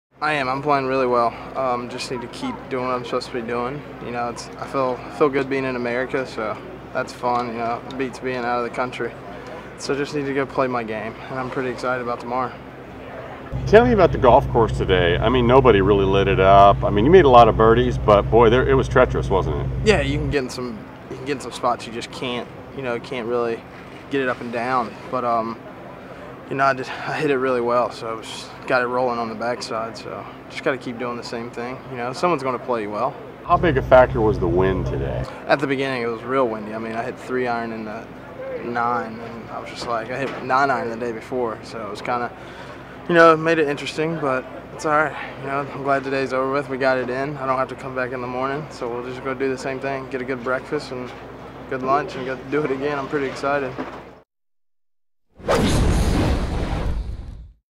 Harold Varner III comments after Round 2 of Chitimacha
Following his second-round 67 at the 2015 Chitimacha Louisiana Open presented by NACHER, Harold Varner III reflects on his play.